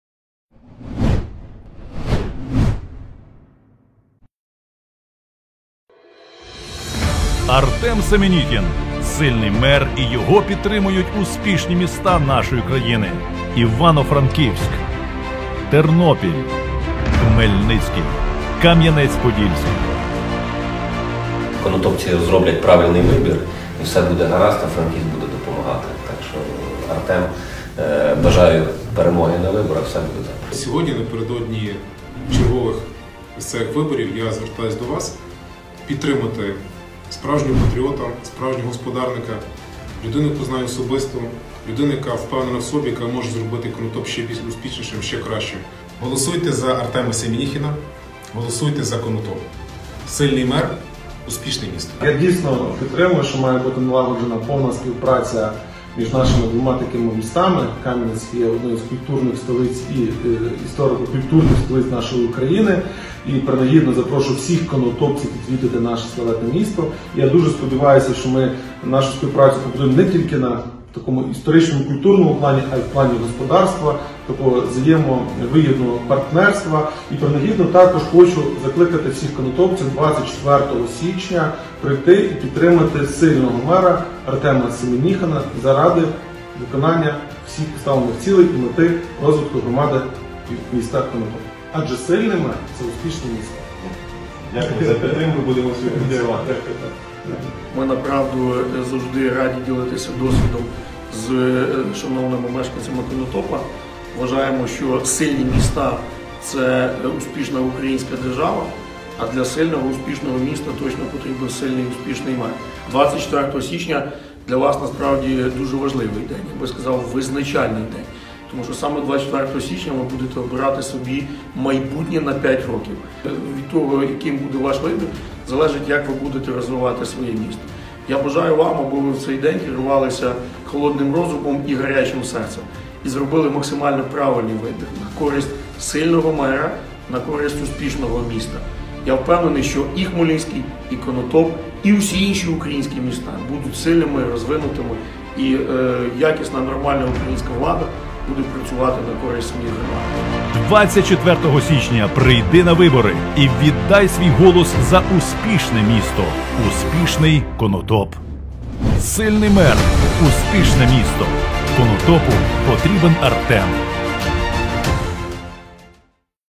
Конотопу потрібен Артем Семеніхін: Сильний мер — успішне місто!
Нового міського голову в Конотопі обиратимуть 24 січня 2021 року. Виборча кампанія стартувала 5 грудня. Нагадаємо, на виборах 25 жовтня переміг Олександр Луговий з результатом у 37,34% голосів виборців, другим йшов чинний міський голова, свободівець Артем Семеніхін — із 31,8%. Луговий помер 16 листопада. До цього він лікувався і не встиг написати заяву про згоду отримати мерські повноваження. Тож 24 січня конотопці знов обиратимуть міського голову.

Артем Семеніхін: Сильний мер — успішне місто!
Конотопу потрібен Артем!